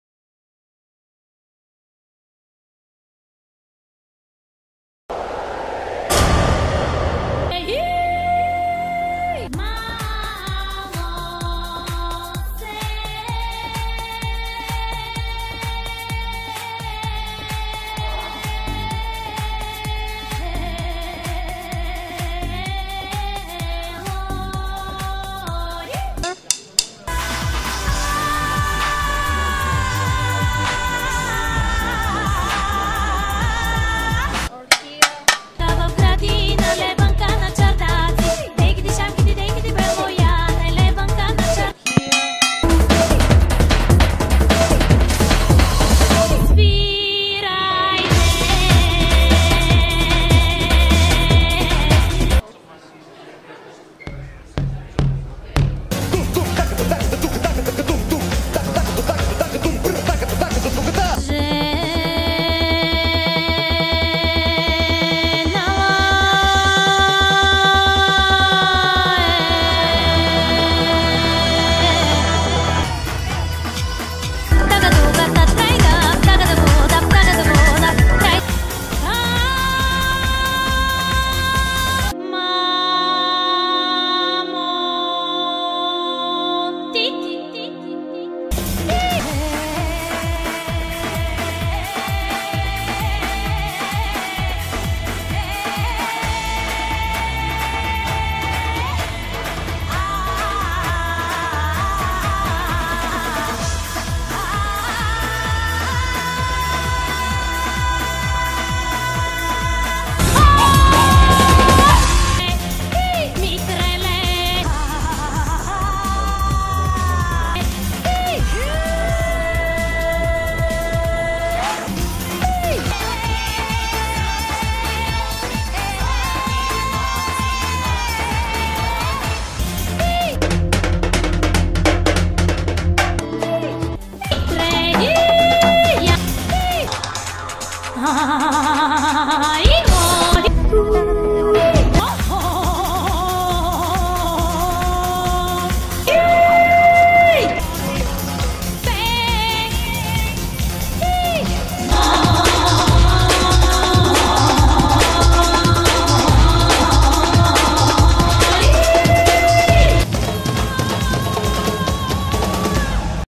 elitsa todorova being noisy for nearly 3 minutes straight
Elitsa Todorova inspired me to go buy a set of ear plugs I'm not gonna lie
1 like= 1 "HEE"